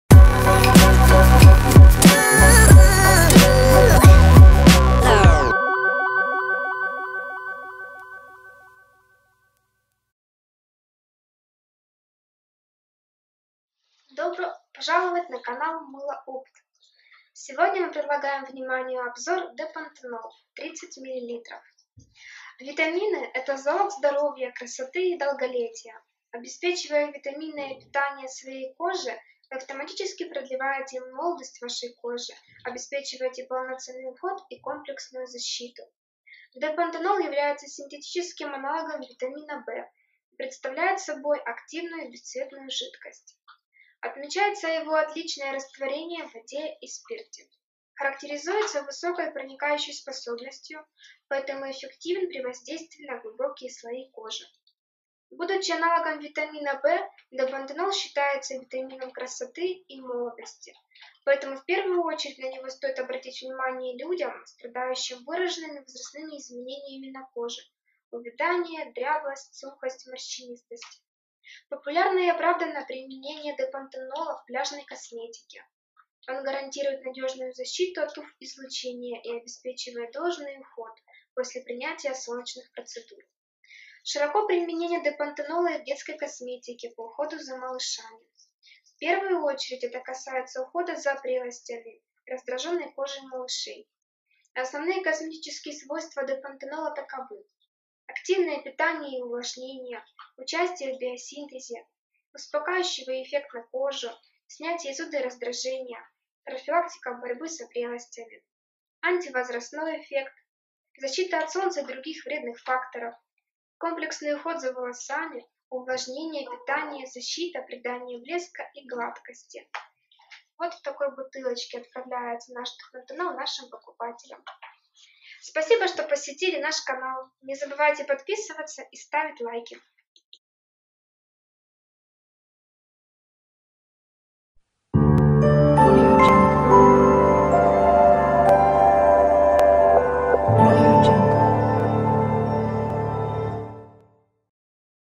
DIY: D-pantenol в косметике//Провитамин B5 для волос// Домашний шампунь
Видео обзор Д-пантенол от магазина Мыло-опт